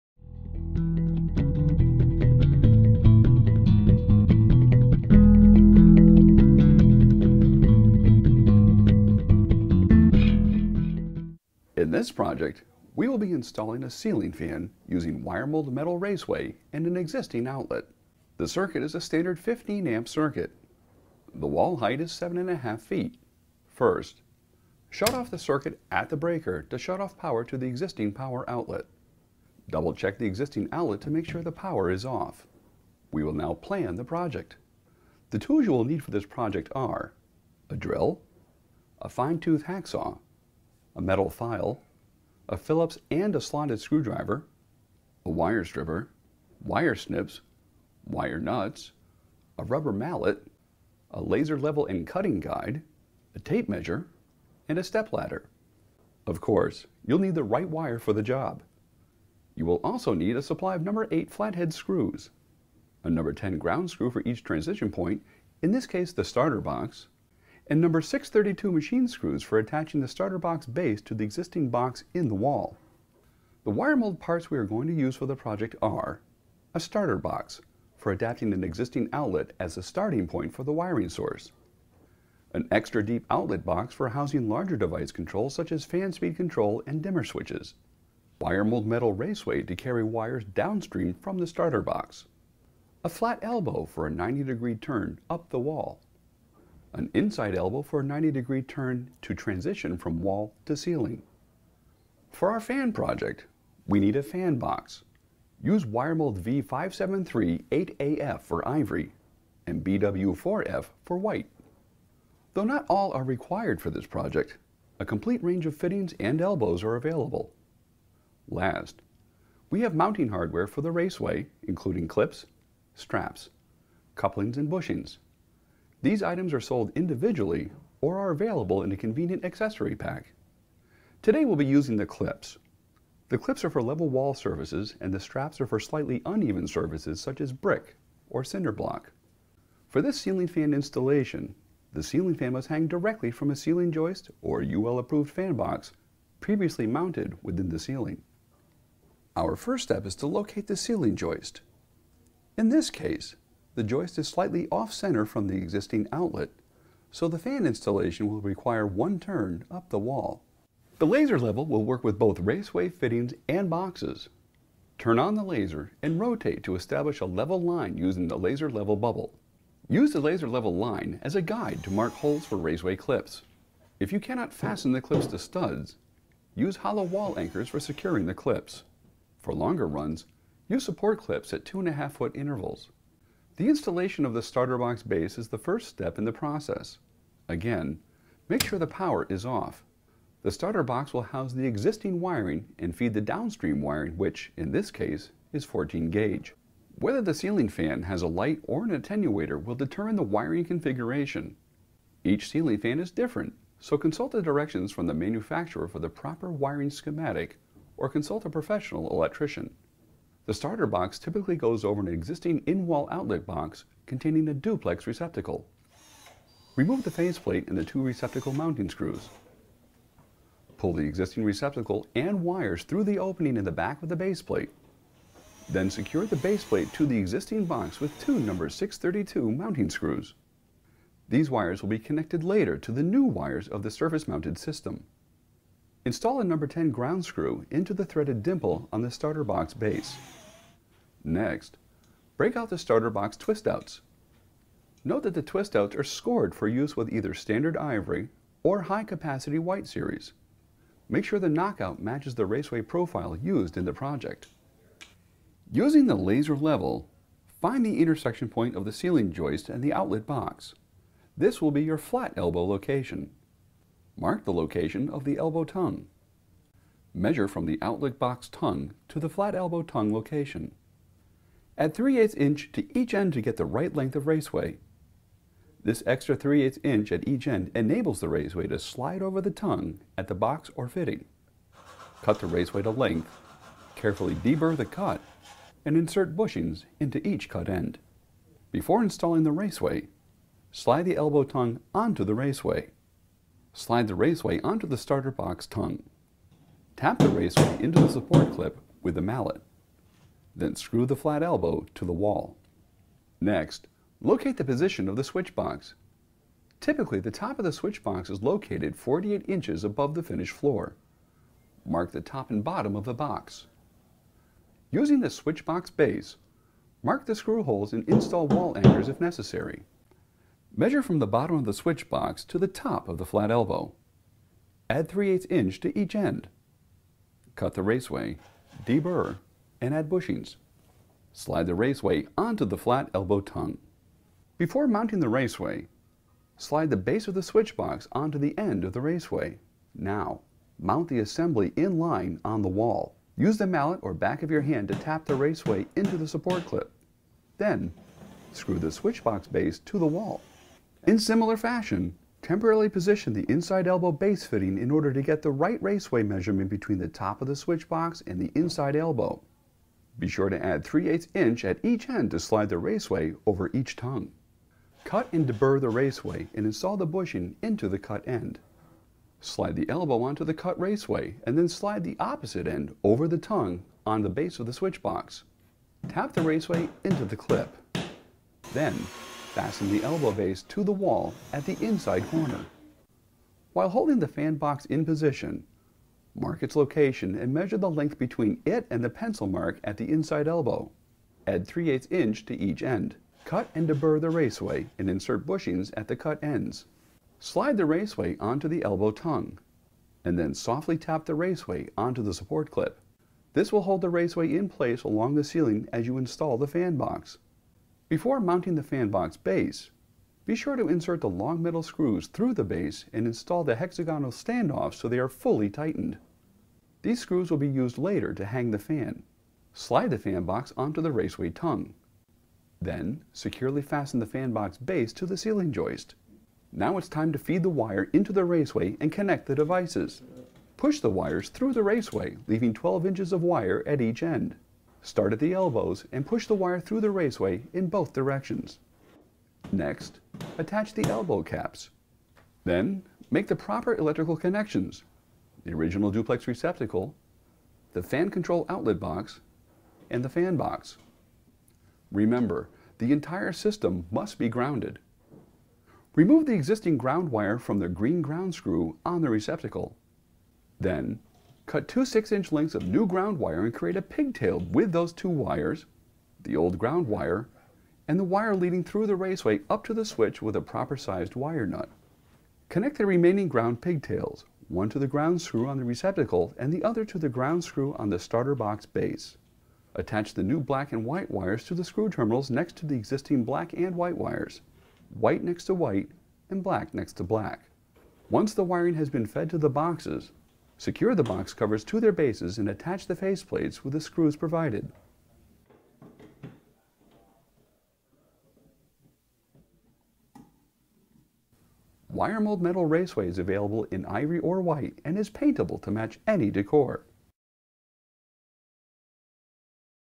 Wiremold: How to Install Metal Raceway
Wiremold Metal Raceway is perfect for heavy-duty applications including: brick walls, concrete, or garages. Available in high or standard capacity, the paintable Metal Raceway makes it a snap to run power wherever you need it. Learn how to install.